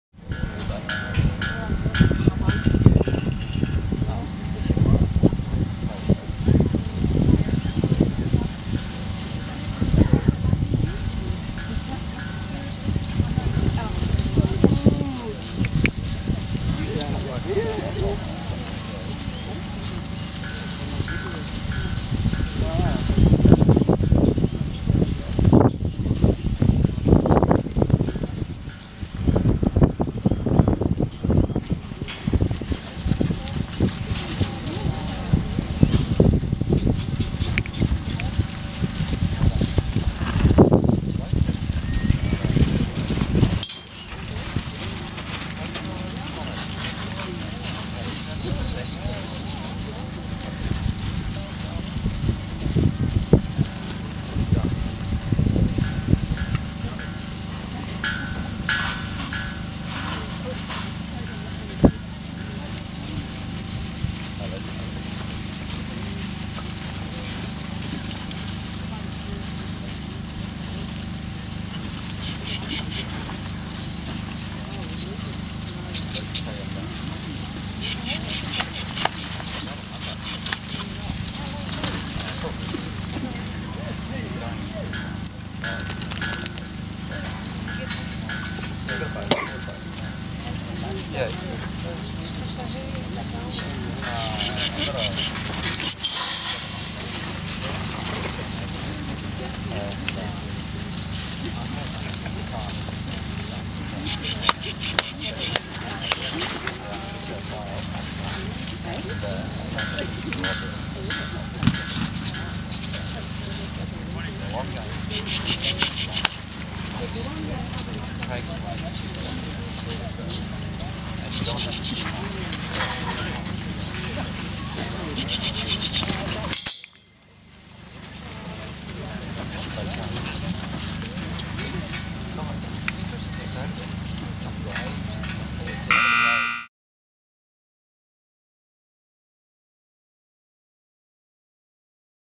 StElmo Georgie Girl & Penny Hick
Penny Hick and STElmo Georgie Girl in the Mitchell Livestock Snaffle Bit Cutting 2013 in Julia Creek.